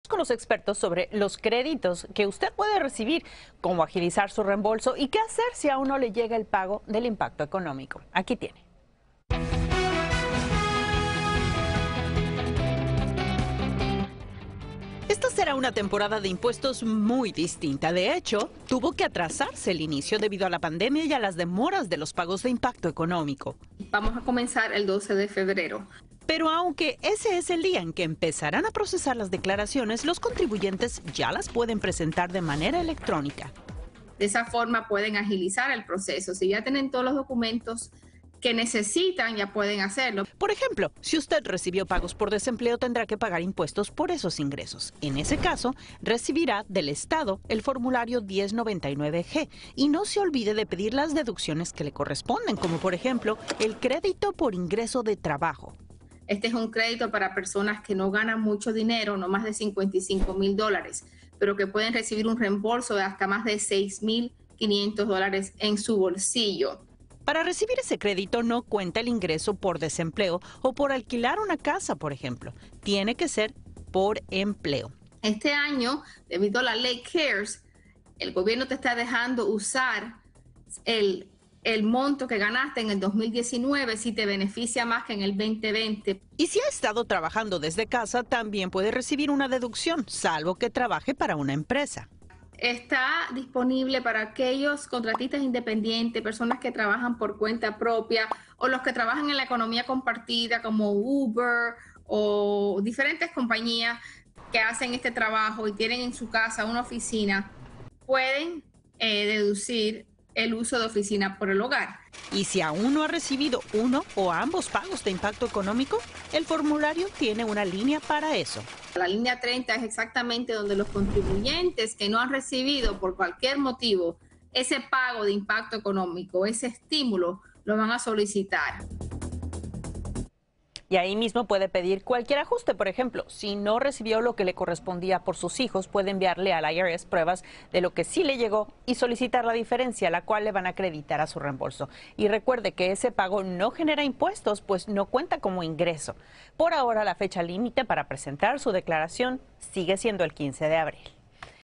Créditos Para los Impuestos Tributarios 2020
Azalea Iñiguez nos tiene información importante para su preparación de impuestos.

Aunque el 12 de febrero es el día que se comenzarán a procesar las declaraciones los contribuyentes ya pueden presentarlos de manera electrónica.

Aquí le dejamos esta información.